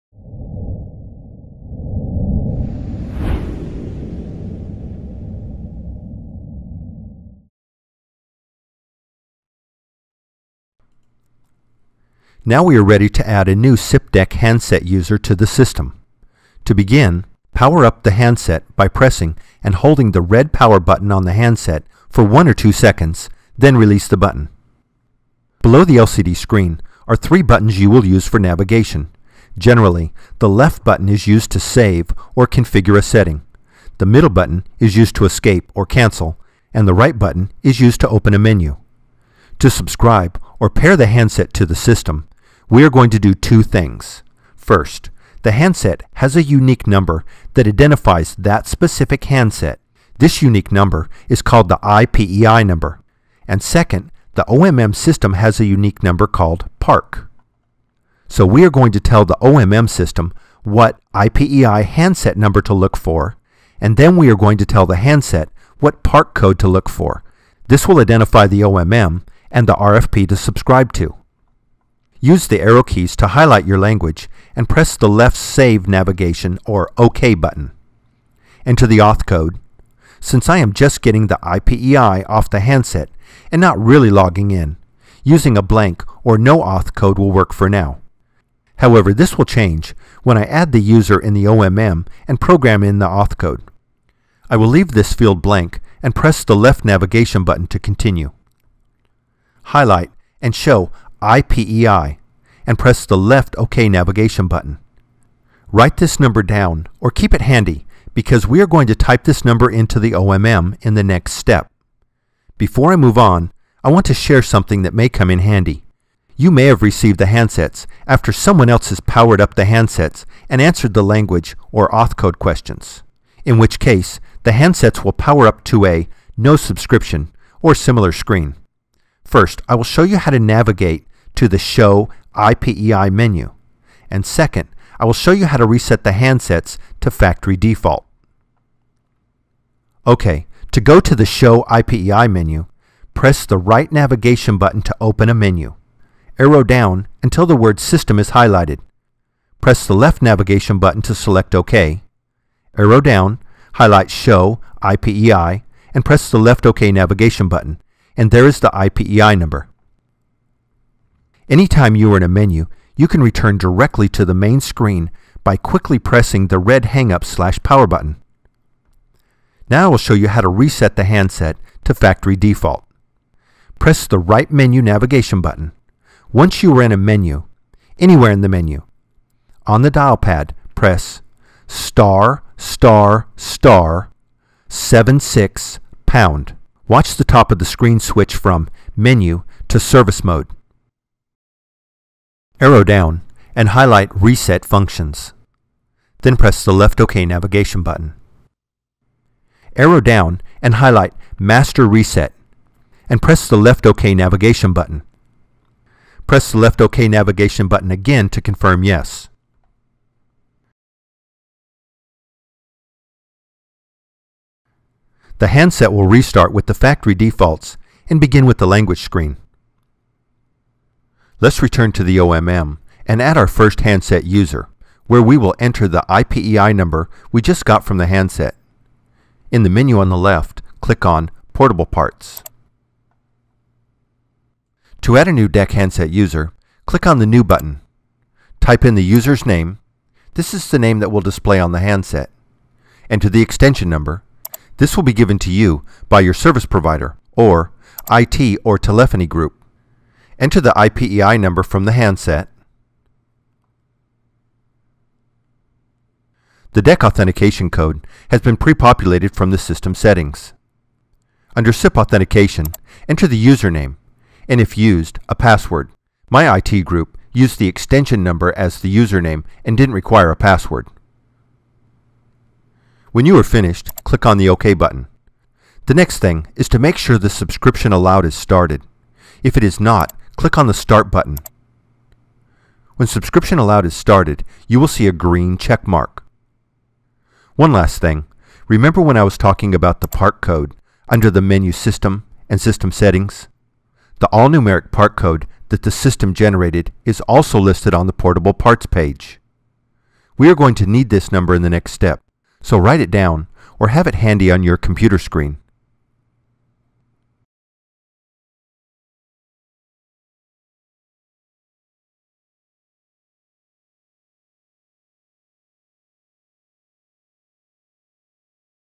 Mitel® SIP-DECT™ Enterprise Basic Installation Mod 5 Add SIP-DECT™ Handset Users
This tutorial is Module 5 in an 8 part series and is intended for a technician or a technical person wanting to learn how to setup, configure and use the Aastra SIP-DECT™ Enterprise solution. This tutorial demonstrates how to add SIP-DECT™ handset users.